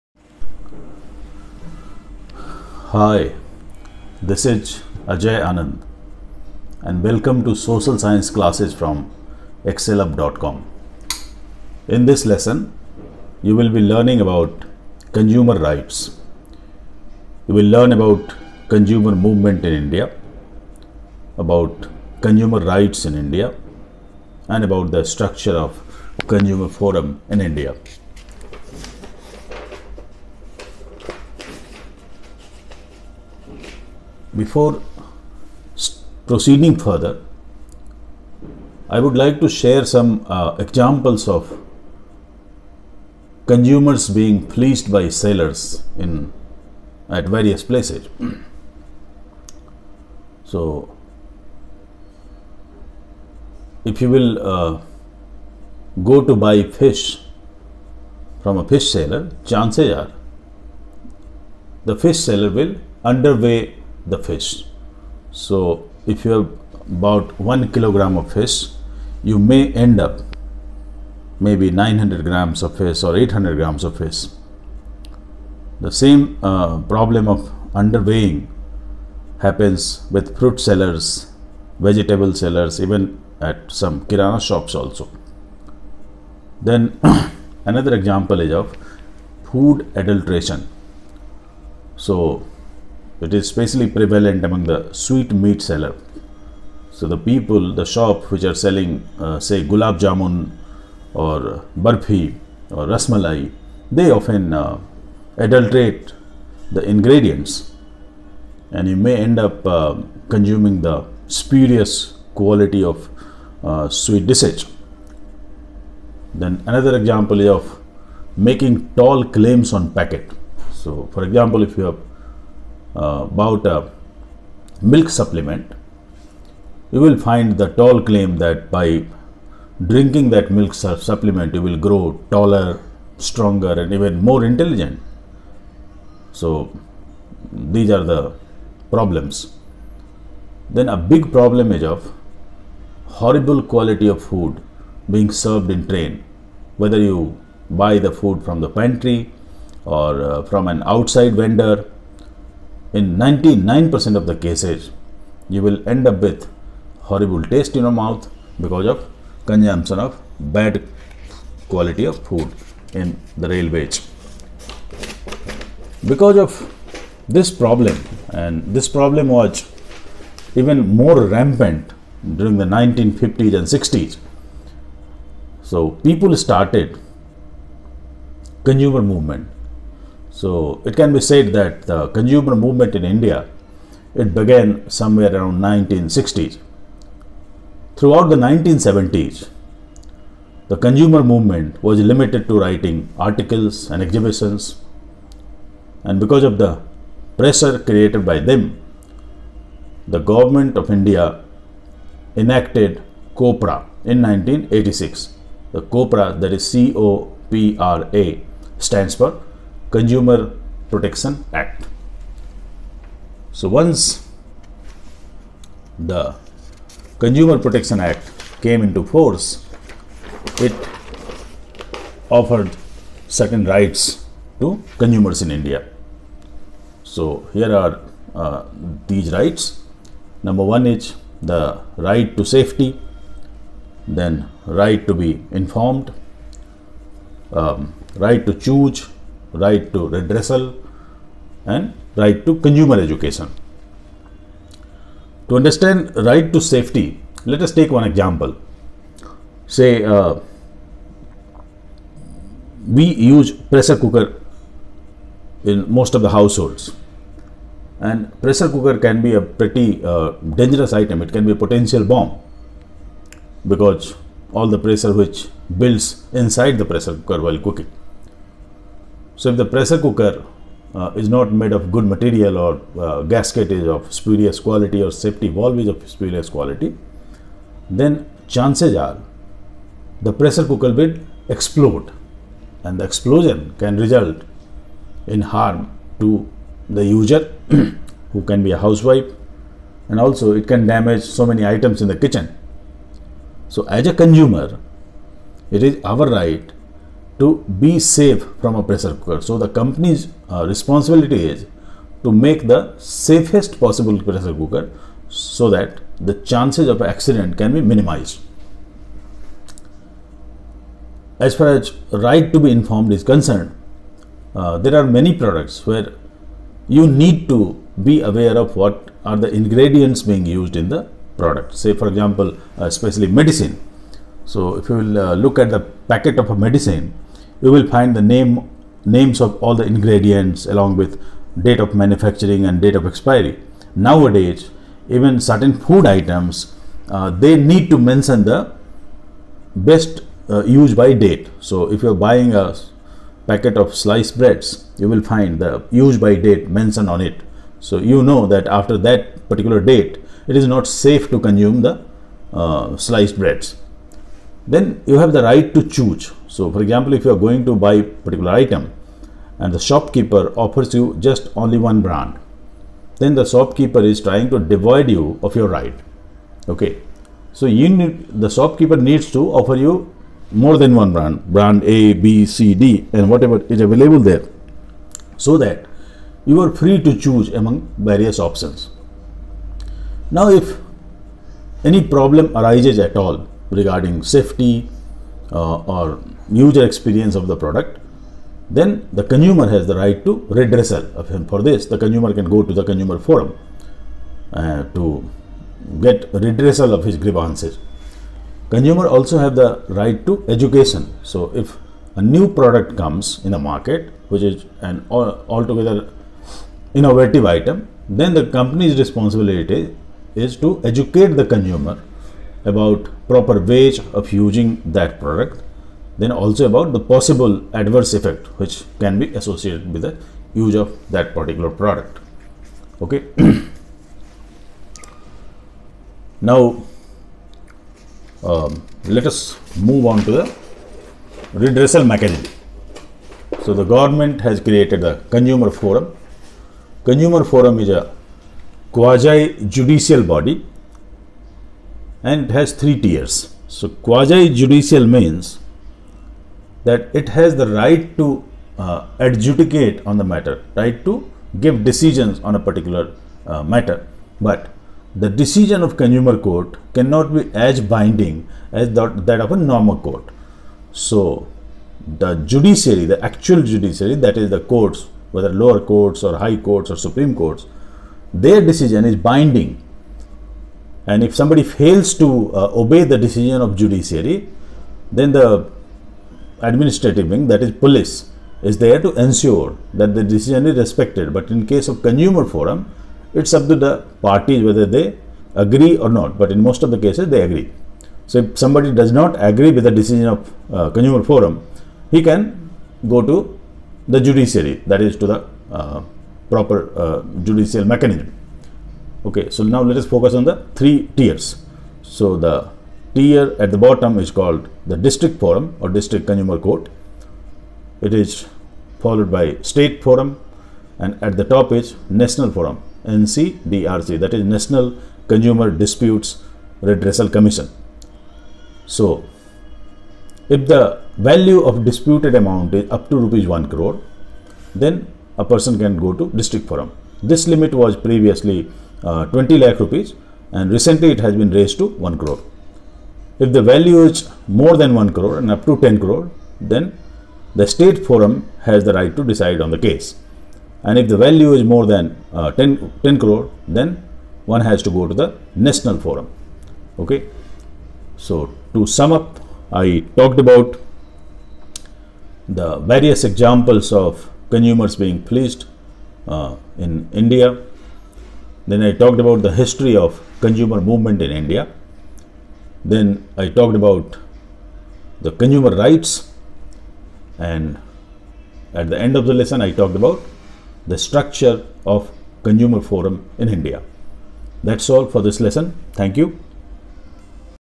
Consumer Rights
Consumers are generally at the receiving end because they are powerless against the mighty business organisation. Consumer movement in India began in the 1960s  and resulted in enactment of COPRA 1986.